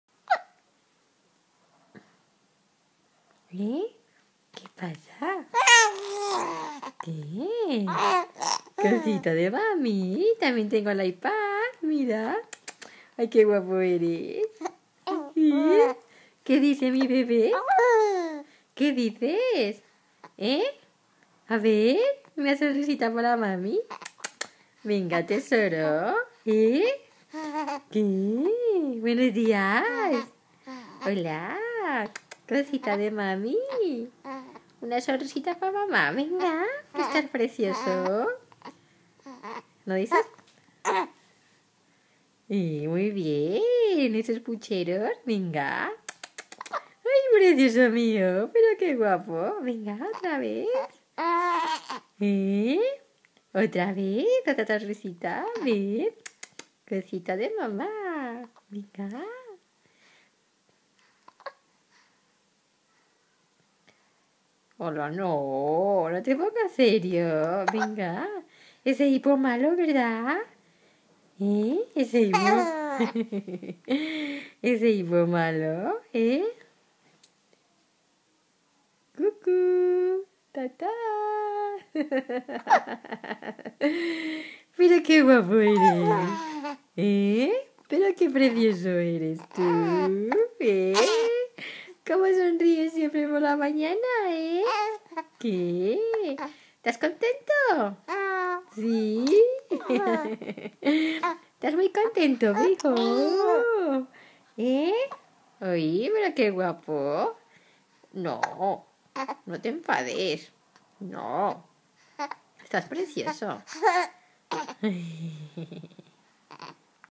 Estimulación bebe de 2 meses!!!
Mi bebe con 2 meses y como se sonrie!